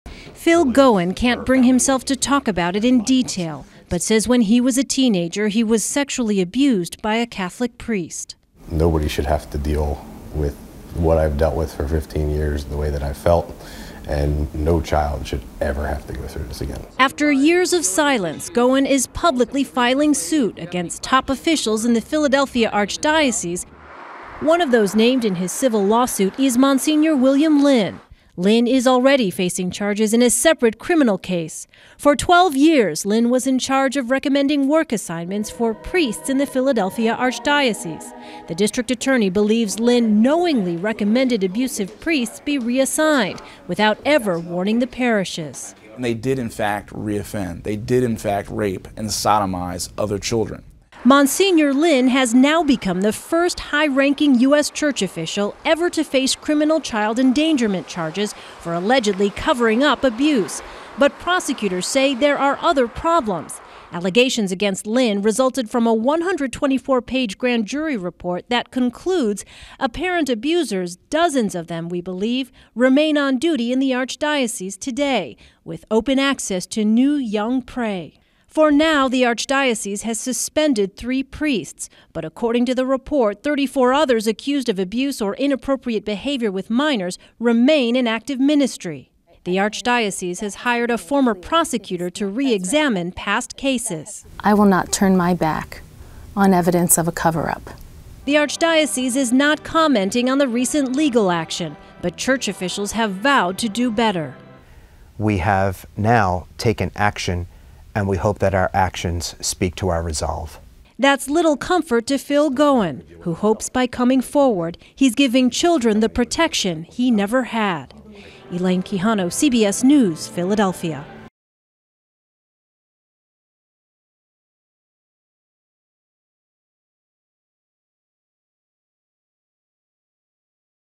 Archdiocese facing multiple lawsuits for child abuse
A new lawsuit is being filed against top officials in the Philadelphia Archdiocese for allegedly failing to protect children against predator priests. As Elaine Qujano reports, this isn't the first suit facing an Archdiocese official.